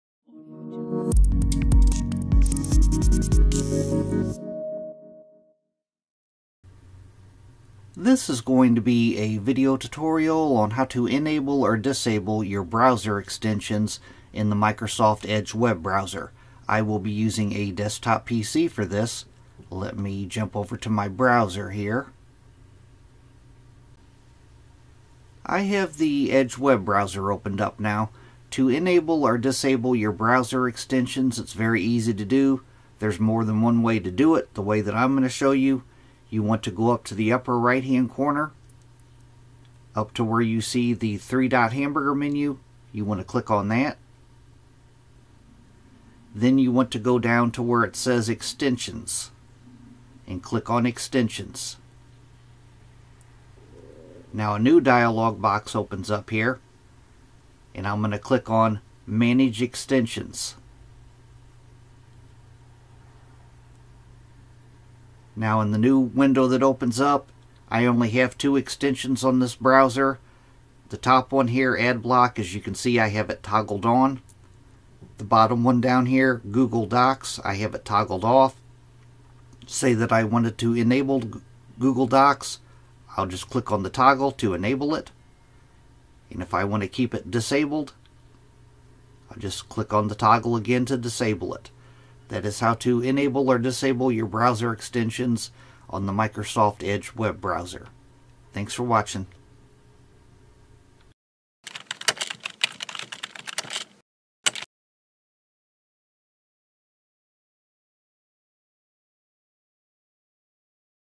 How To Enable Or Disable Extensions On The Microsoft Edge Browser | PC Tutorial 👌
💻 How to Enable or Disable Extensions in Microsoft Edge on PC: Easy Step-by-Step Guide! 👌

Welcome to our tutorial on how to enable or disable extensions in the Microsoft Edge browser using your PC! Extensions can enhance your browsing experience, but there are times when you may need to manage them for better performance or troubleshooting. In this step-by-step guide, we'll show you how to quickly enable or disable extensions in Microsoft Edge.

Simple Steps
1. Open The Microsoft Edge Browser.
2. Click On The 3 Dot Hamburger Menu In The Upper Right Hand Corner.
3. Select "Extensions" from the dropdown.
4. In the dialog box that opens, click on "Manage Extensions".
5. In the center pane search for the extension that you want to turn on or off and click on the toggle.

Chapters
0:00 How To Turn Off Or On Extensions In Microsoft Edge
0:20 Open The Microsoft Edge Browser
0:37 Click On The 3 Dot Hamburger Menu
0:47 Click On Extensions
0:57 Click On Manage Extensions
1:07 In The Center Pane Find The Extension You Want To Turn On Or Off
1:17 How Toggle On Or Off An Extension